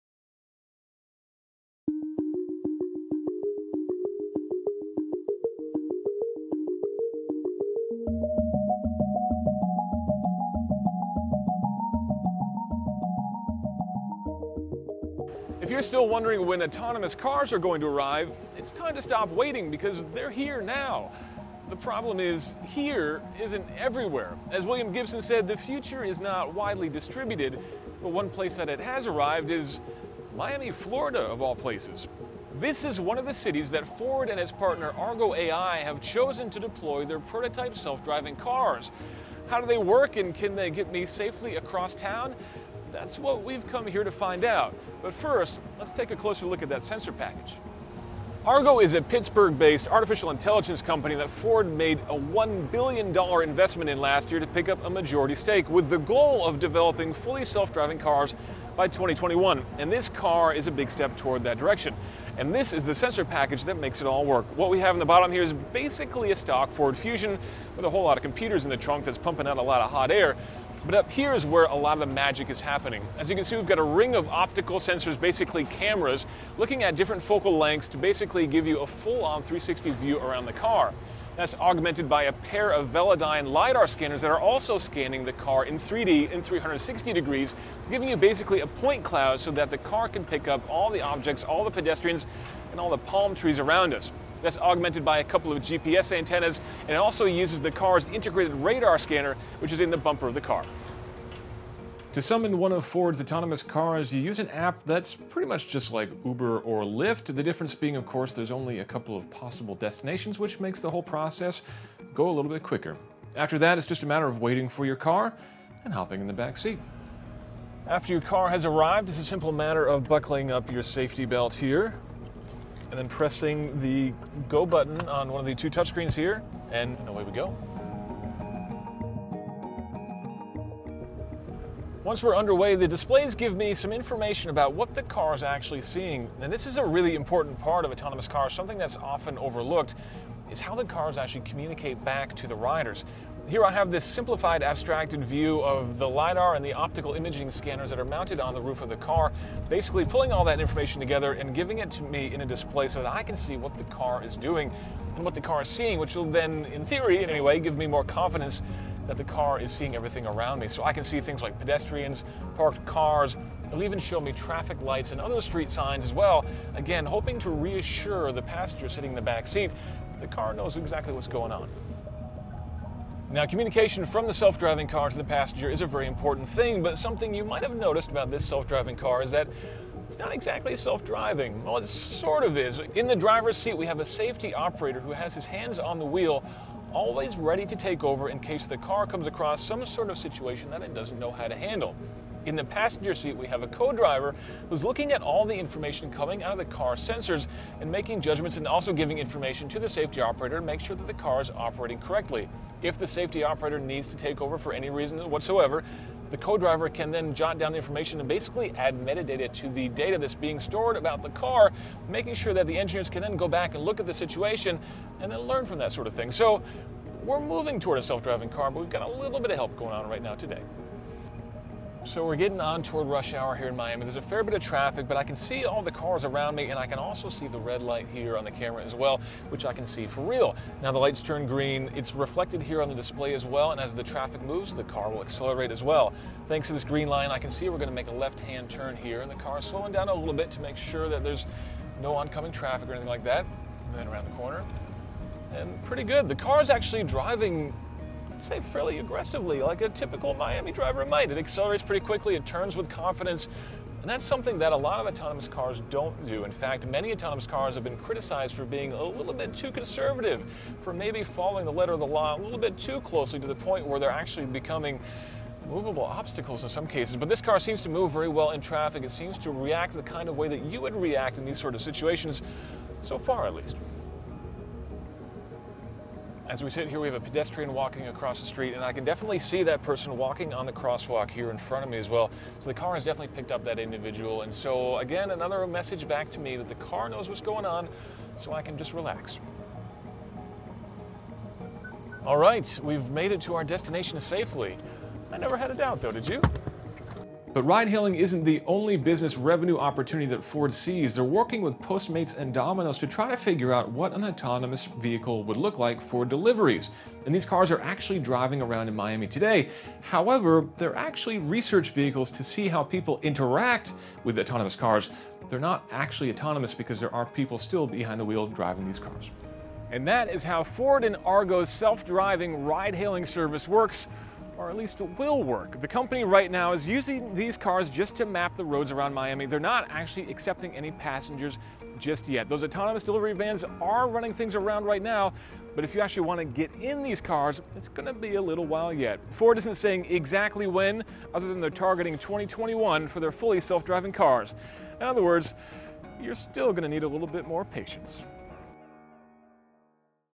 Touring Miami in Ford's prototype driverless car
Self-driving cars aren't quite ready for prime-time, but we're getting close and Ford's work with Argo AI in Miami is helping get us there. Join us for a ride in Ford's prototype self-driving car.